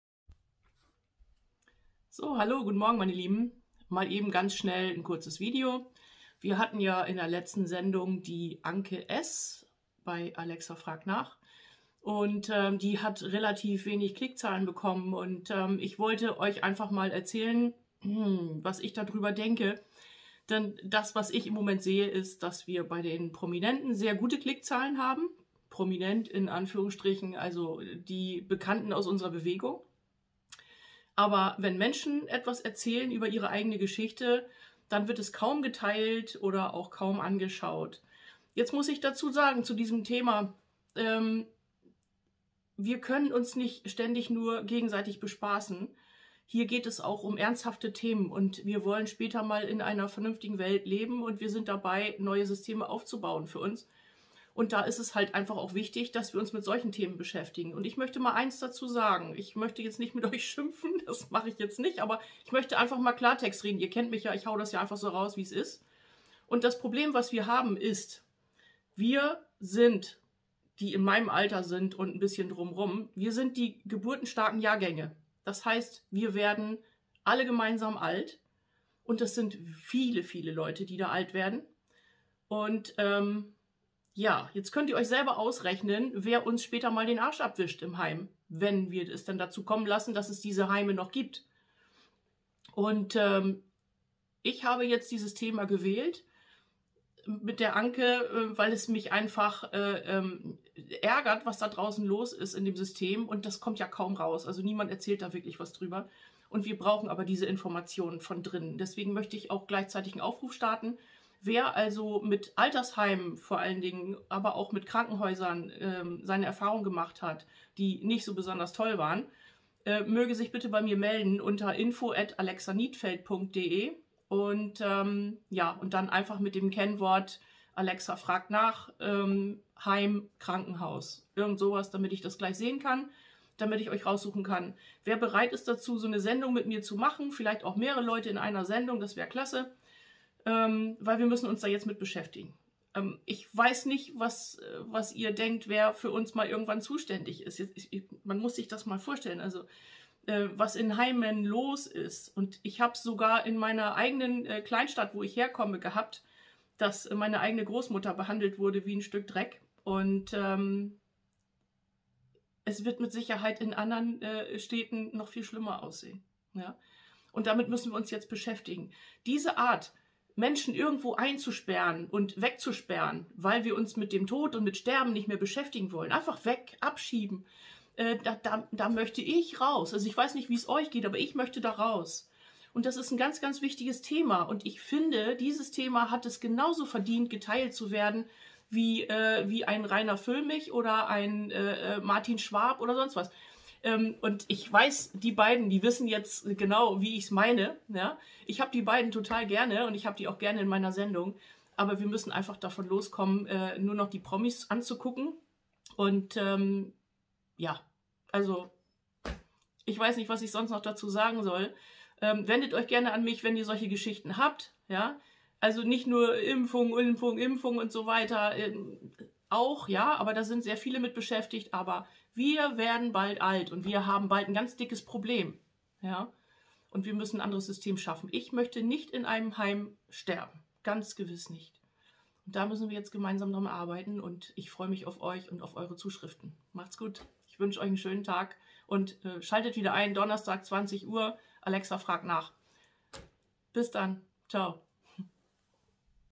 Ein knallhartes Wort an Euch alle und auch an mich selbst. 11.02.23
Zeit zum Umdenken! Erzählt Eure Geschichten!

Die Zeit drängt! Es werden bald so viele Menschen auf einmal alt wie nie zuvor. Auch ich gehöre gerade noch zu den geburtenstarken Jahrgängen. Wir müssen dringend darüber reden und diesen Themen auch eine Chance geben.

Du hast eine schlimme Geschichte zu erzählen, die in einer Klinik oder in einem Seniorenheim passiert ist? Melde Dich bitte unter:

Kennwort: Klinik/Seniorenheim

Eure Alexa